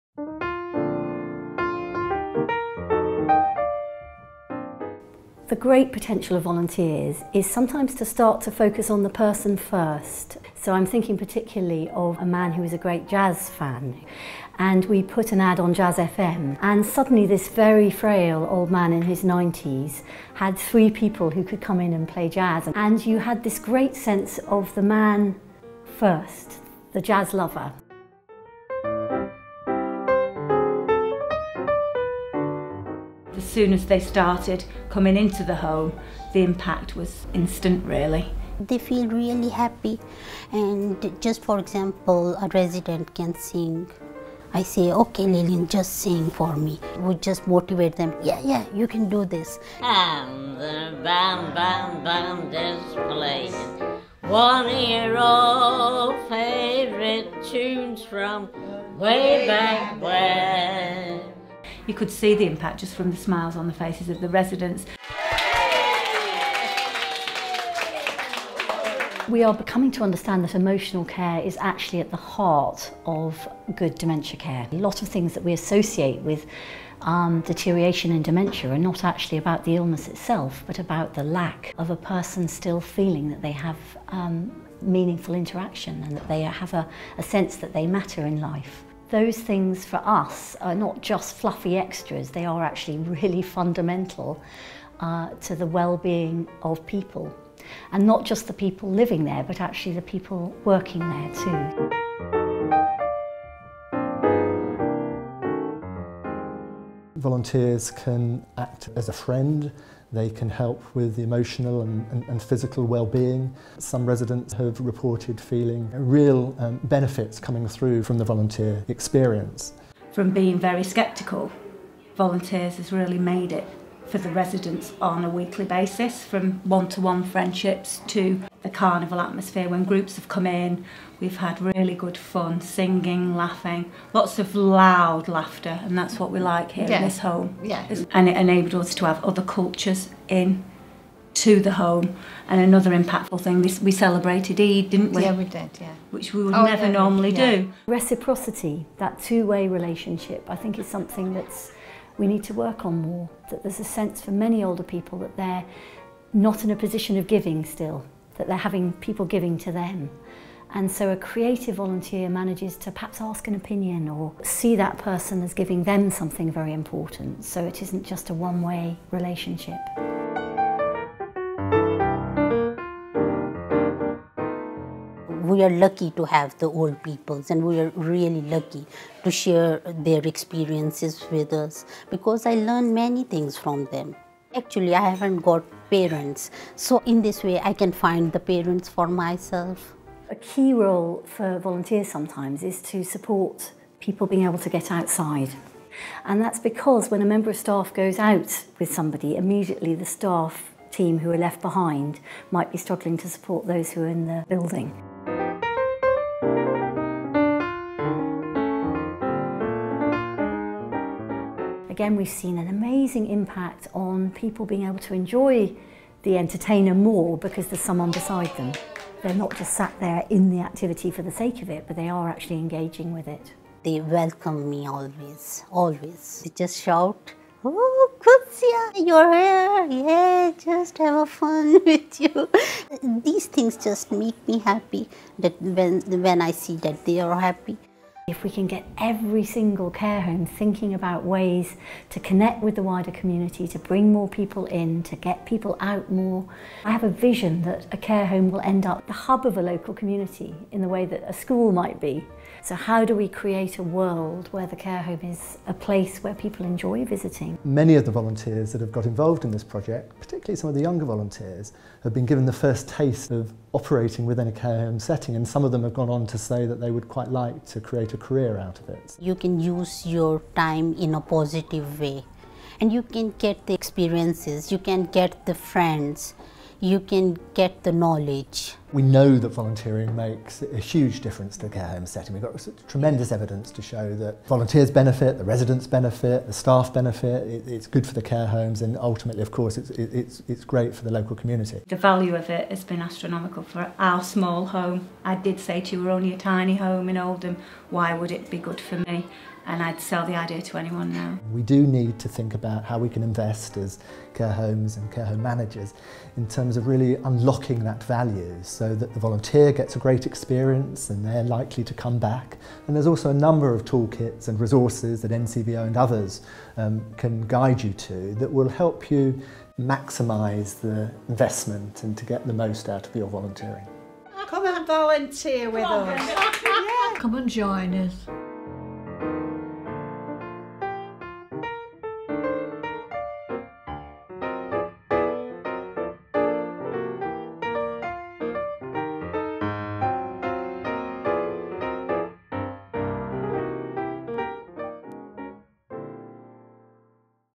Volunteering in Care Homes: Making Connections, Improving Lives
This five minute video captures the impact of volunteering on residents, care home staff and volunteers whilst experts from the fields of dementia care and volunteering reflect on the significance of these findings.

The participants interviewed have all been involved in the Volunteering in Care Homes Project, a three year pilot that brokered relationships between Volunteer Centres and their local care homes to recruit and support volunteers  to fulfil roles that enabled older residents to engage in activities of their choice.

The video incorporates the key findings  from the project evaluation and has been produced to promote volunteering in care homes and the resources produced by the project.